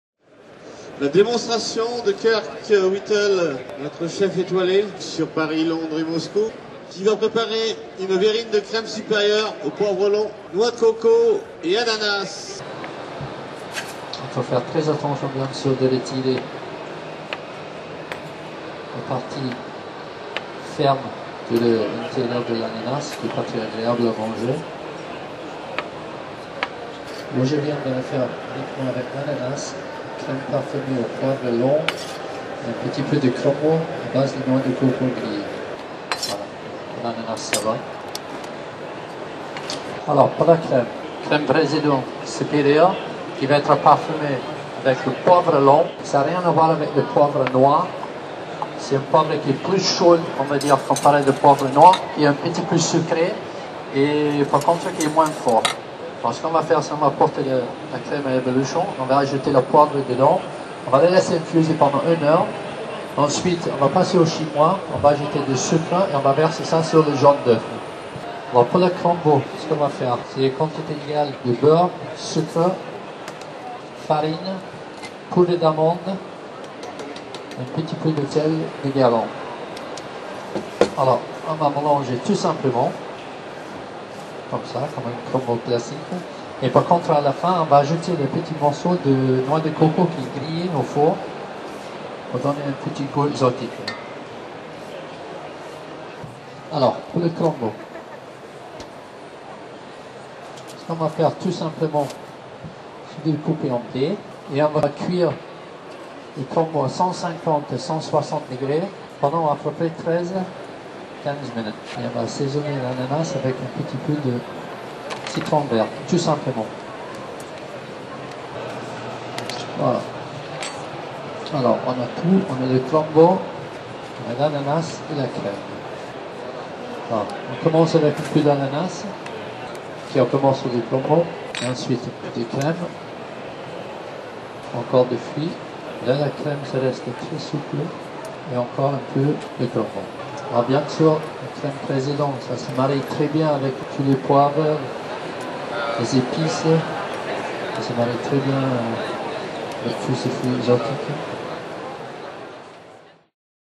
Recette de Verrine à base de Crème Supérieure par Kirk Whittle
Découvrez la recette "Verrine de Crème Supérieure au Poivre long, Ananas et Crumble Noix de Coco" réalisée par Kirk Whittle, Chef Pâtissier étoilé - Paris 6ème sur le stand Président Professionnel lors du salon Europain.